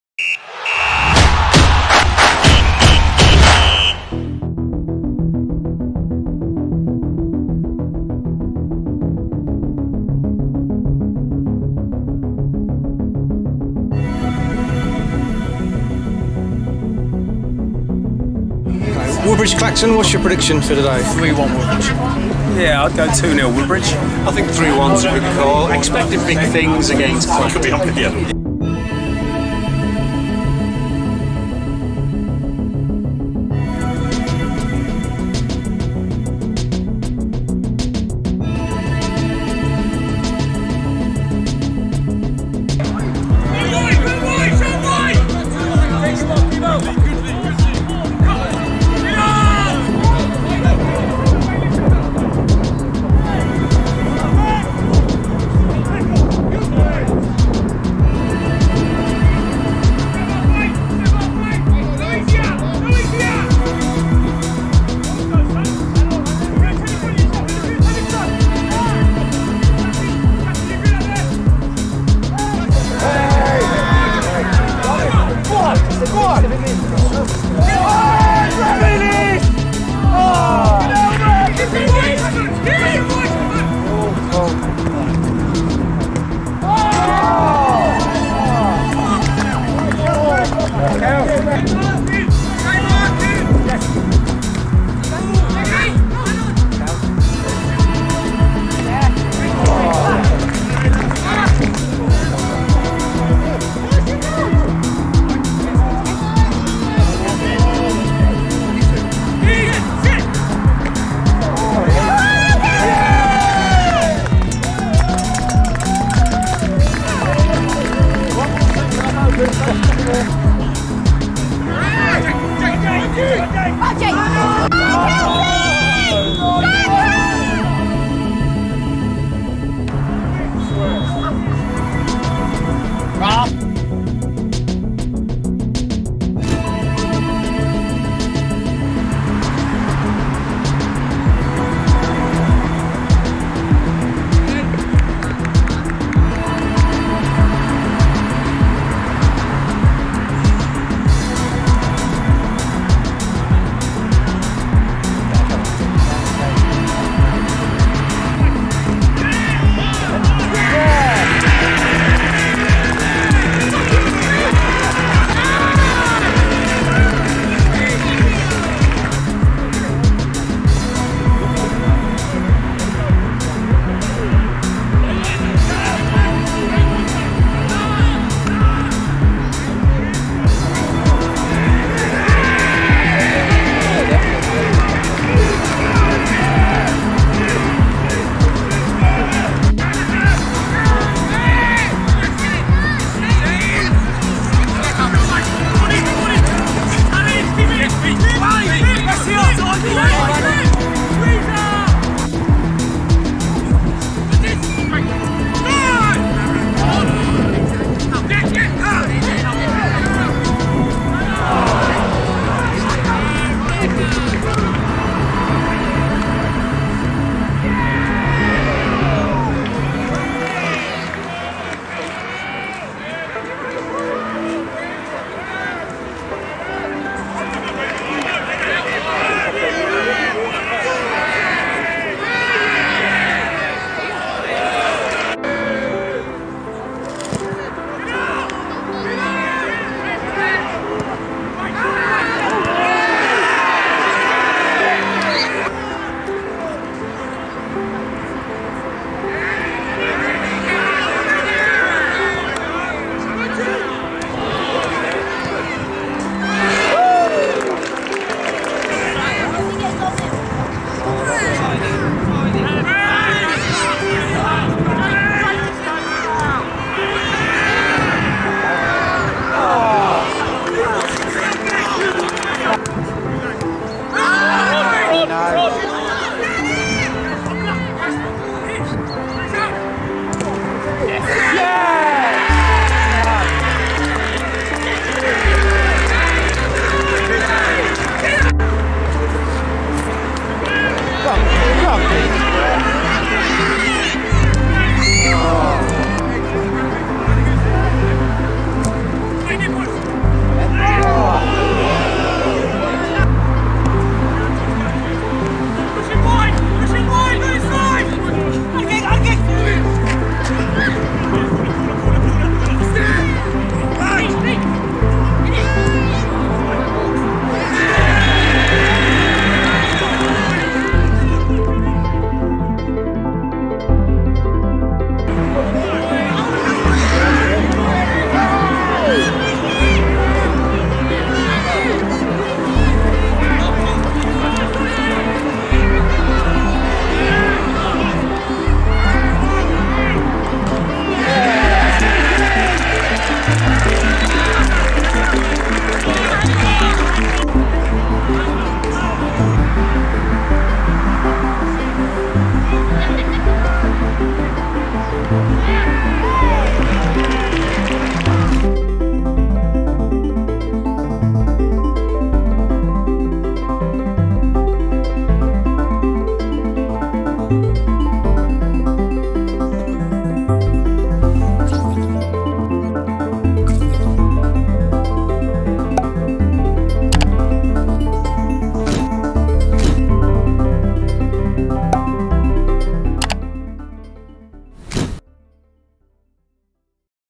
Woodbridge Town vs FC Clacton (4-2) | Thurlow Nunn Premier League Match Highlights
Woodpecker TV brings you highlights from Woodbridge Town's victory over FC Clacton in The Thurlow Nunn Premier Division.

194 Fans watched the match which included a Luke Mallet making his debut after coming on at half time and marking it with a goal!

Kelsey Trotter capped off a great performance with 2 goals and was unlucky not to get a hatrick had it not been for some off-side decisions (which looked the correct call).

Ryan Keeble scored the 4th Woodbridge goal whilst the FC Clacton goals were scored by Karl Andrade and Harry McDonald.

It also helps the exposure of the channel and videos

Special Thanks to Paul Leech for the photo used in the YouTube thumbnail.